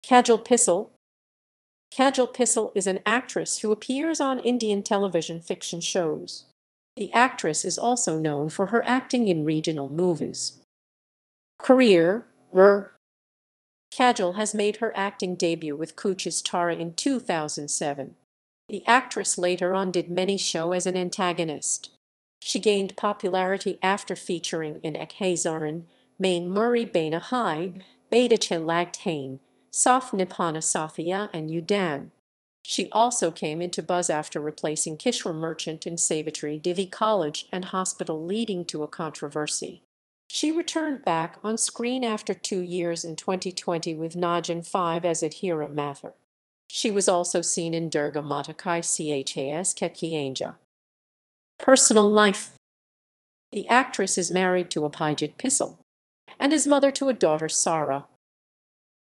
Kajal Pisal
Kajal Pisal is an actress  who appears on Indian television fiction shows. The actress is also known for her acting in regional movies.